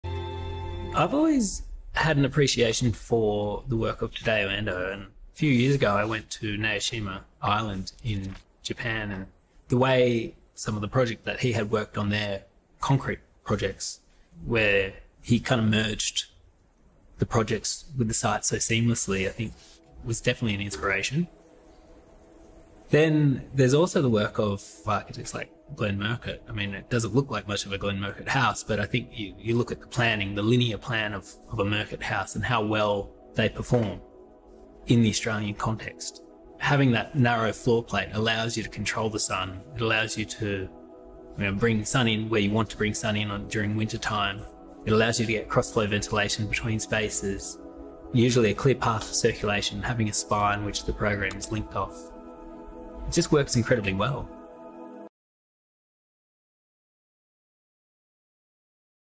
Tadao Ando & Glenn Murcutt's Influence on Wallis Lake House
Wallis Lake House - architect Matthew Woodward names several key influences on his work.

Sign up to our mailing list and we'll send you a selection of FREE award winning films (including this one) exclusive to Shelter!

Whether it’s the lake-glass of sunset pinks, the salt spray ocean mists rolling over the Booti Booti National Park, or the expanse of sky horizons with Australian bush foreground, the Wallis Lake House is designed to capture it all.

Architects: Matthew Woodward, Callum Eve, Patrick Maitland, Nicholas Papas - Matthew Woodward Architecture